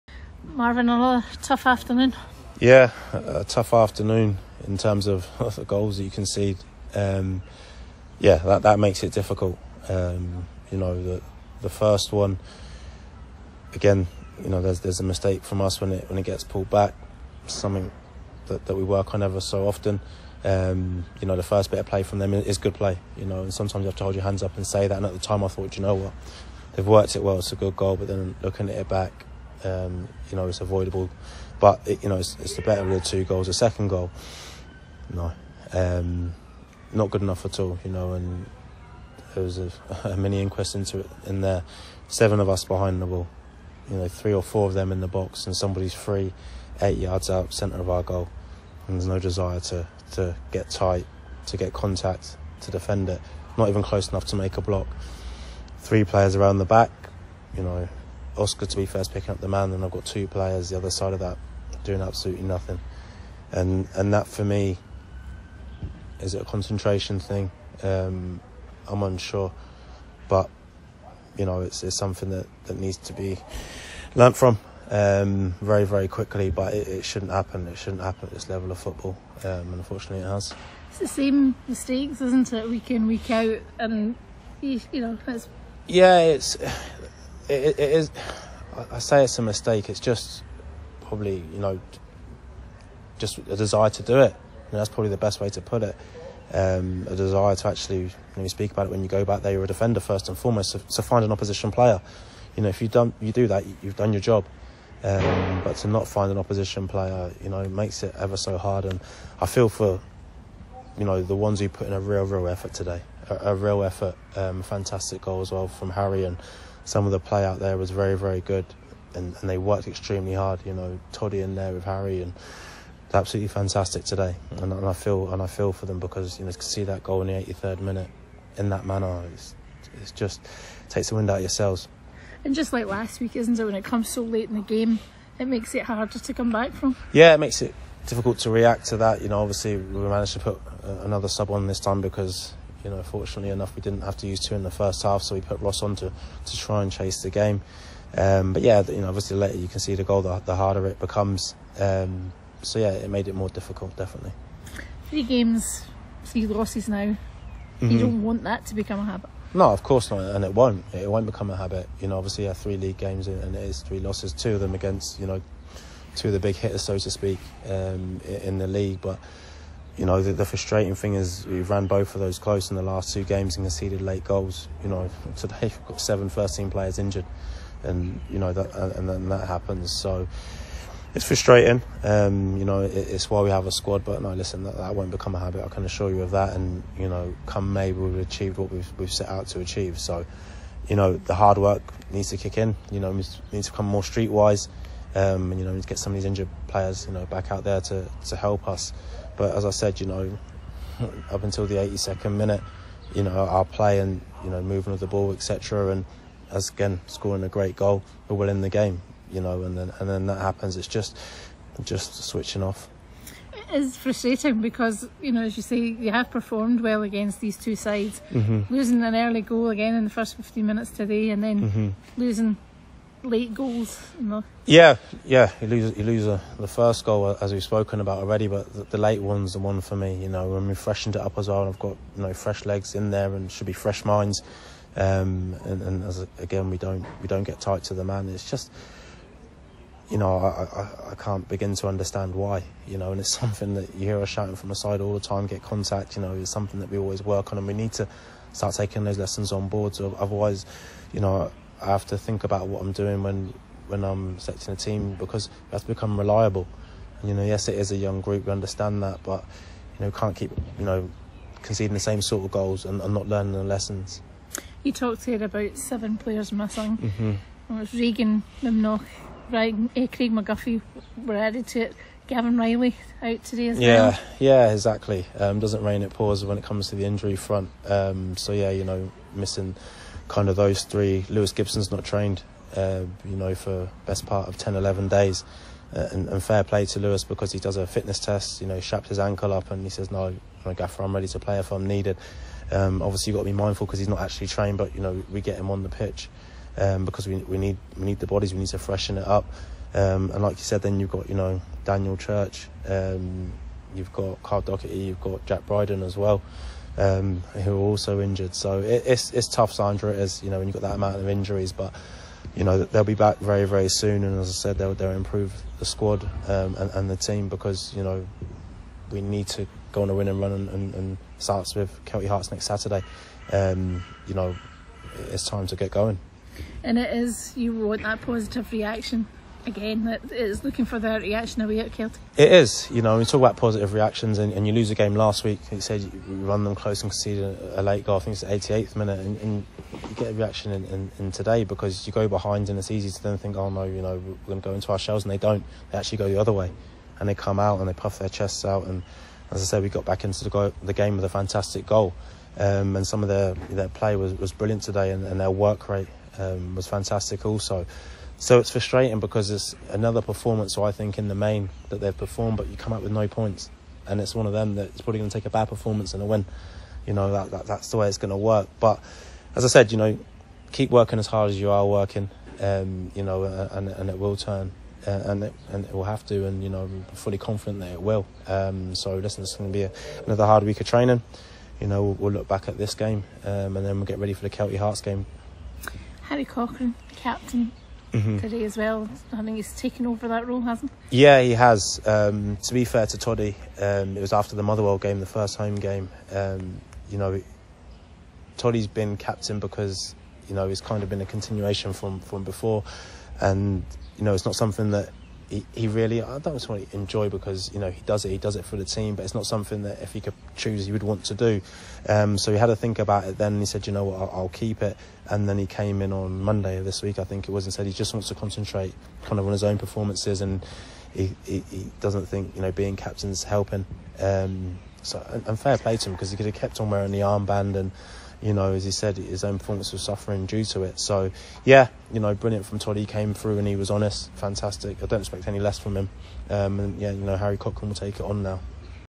Marvin Bartley | Hamilton | 23 Sep 2023
The manager gave us his post-match views.